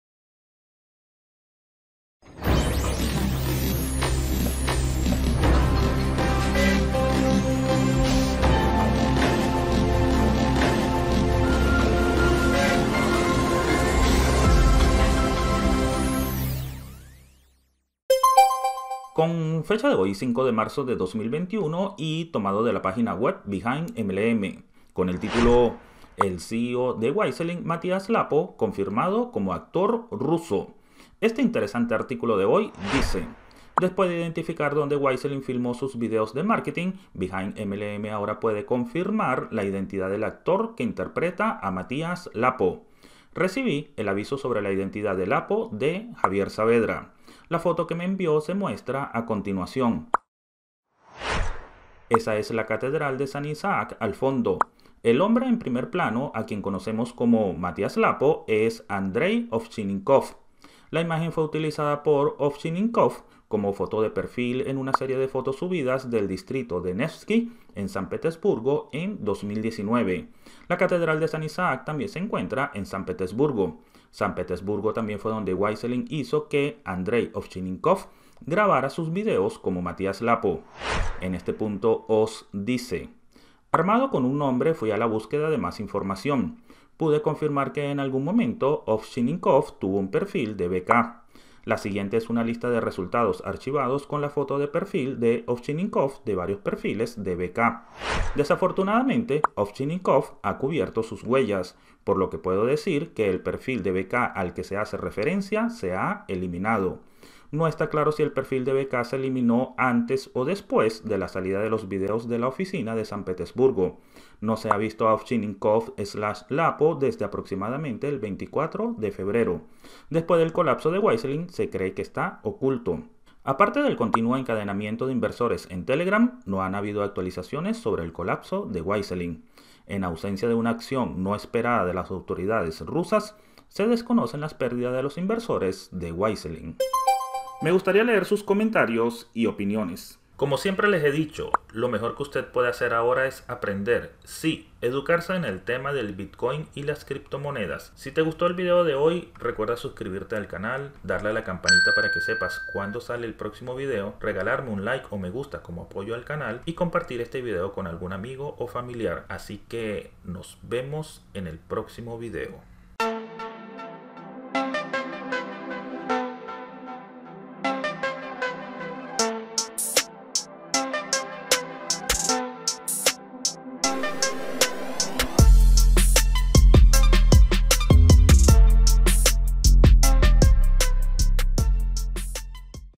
WISELING — 📢 CONFIRMADO "MATIAS LAPPO" COMO ACTOR RUSO 💸
WISELING — Confirmado que "Matias Lappo" es un actor ruso

🎁 Donaciones y contribuciones para el canal
Billetera BTC: 3QGt2yZZ8enFidw6fYnrbViJR2FPQHevSN
(Solo envíe Bitcoin otras criptomonedas diferentes se pierde el dinero)